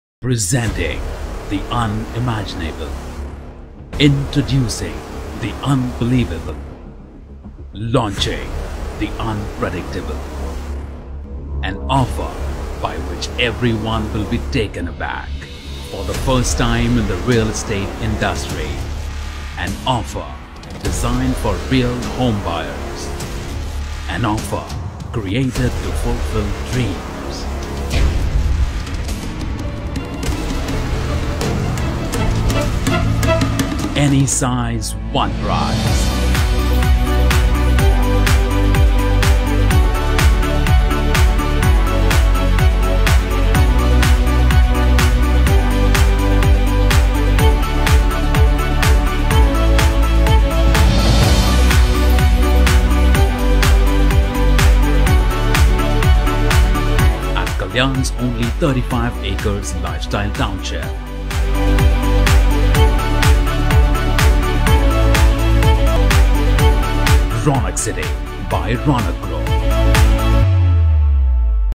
Raunak Group: Any Size. One Price
We've got the most exciting and innovative offer in the history of Indian Real Estate Industry for you! Want to find out what is it about? Come see us at CREDAI MCHI property expo at Kalyan to about - "Any Size, One Price"!